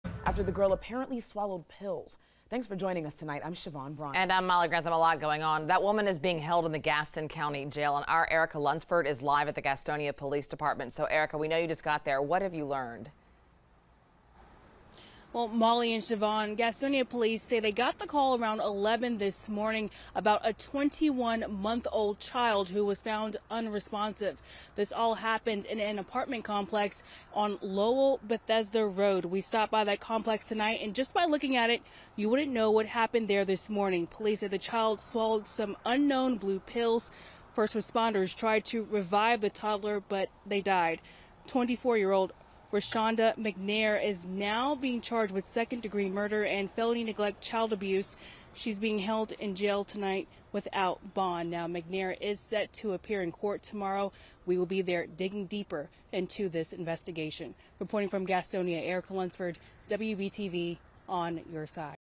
Police: Woman Charged After 21-Month-Old Dies After Swallowing Pills In Gaston County
The girl was found inside an apartment on Lowell Bethesda Road on Tuesday morning.